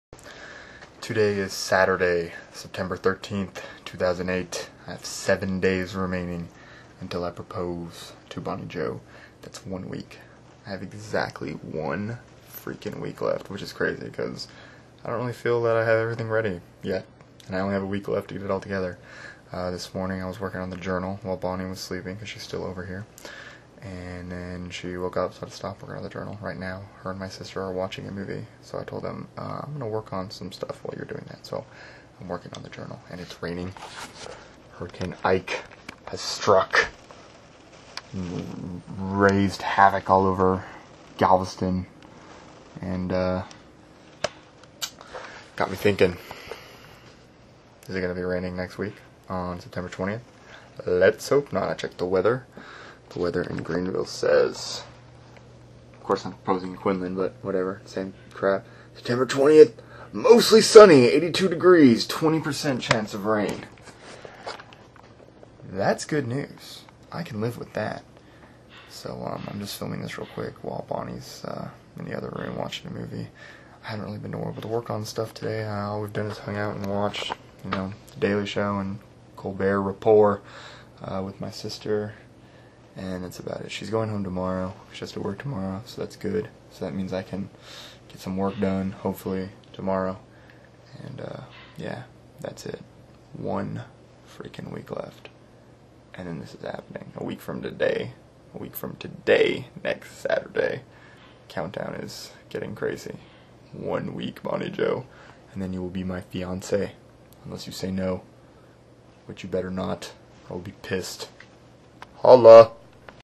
2008-09-13 Video Journal: One Week Left
Saturday, September 13, 2008 - SEVEN DAYS TILL I PROPOSE. I have exactly ONE WEEK LEFT! I didn't get to work on much because Bonny is still over. I did a little work on the journal in the morning before she woke up, and I've been doing a little more work on it while Bonny and my sister watch a movie. I checked the weather for next Saturday and it's supposed to be sunny, 82 degrees with only a 20% chance of rain.